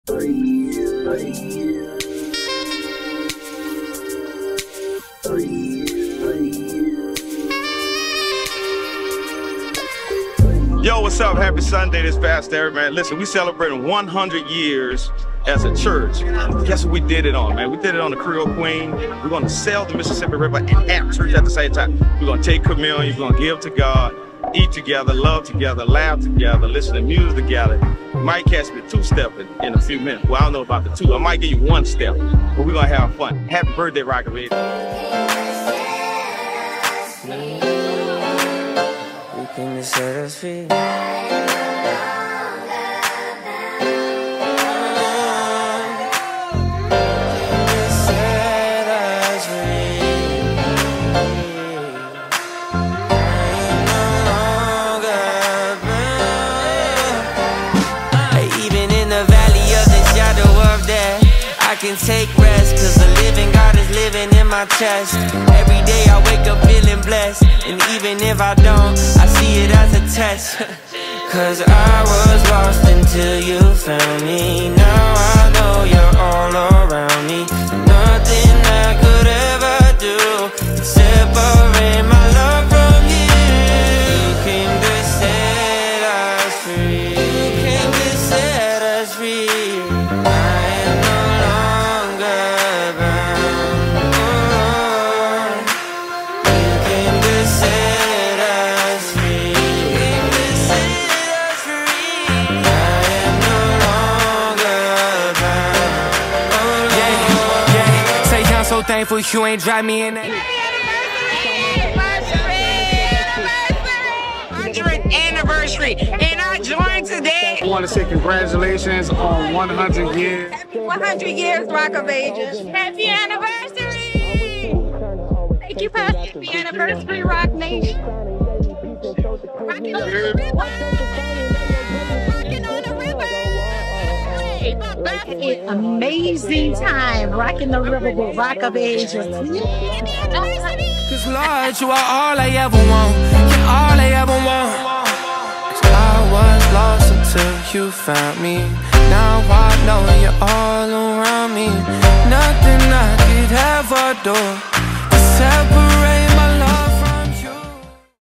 ROCKIN ON THE RIVER 🛥️
AIR | LAND | WATER 💦 ROCKIN THE RIVER

Things don’t turn up in this world 🌍 until somebody turns them up ⬆️. There is no discovery without risk an what you risks reveals what you VALUE.

Thank you 🙏🏾 @rockofagesbc for always following the Vision but doing it with such class. Having a moment to Praise God, Take our Communion and Preaching on the water was AMAZING 🤩

I NEED 1,000 SHARES | Just to Remind the Devil he LOST AGAIN